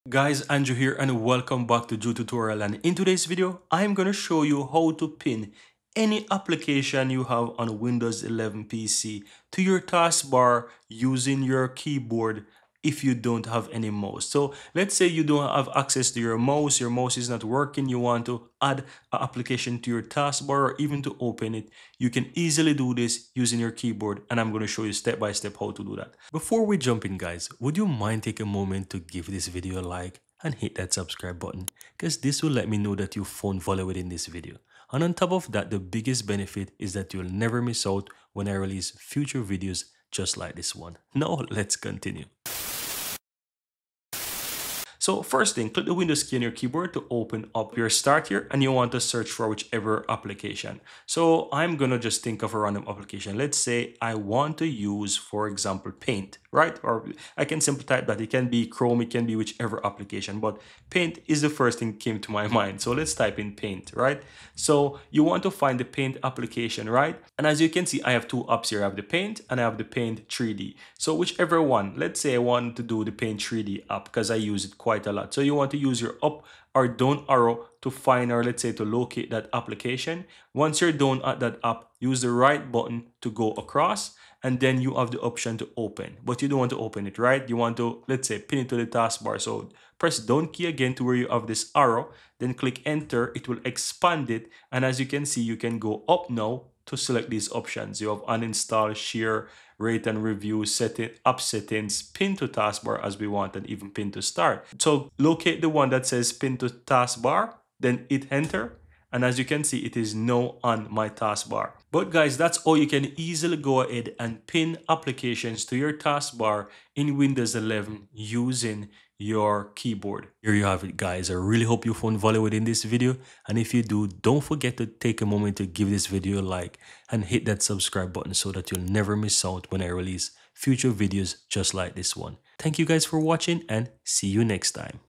How to Pin to ANY Apps to Taskbar WITHOUT MOUSE on Windows 11
In this tutorial, I show you How to Pin to ANY Apps to Taskbar WITHOUT MOUSE on Windows 11.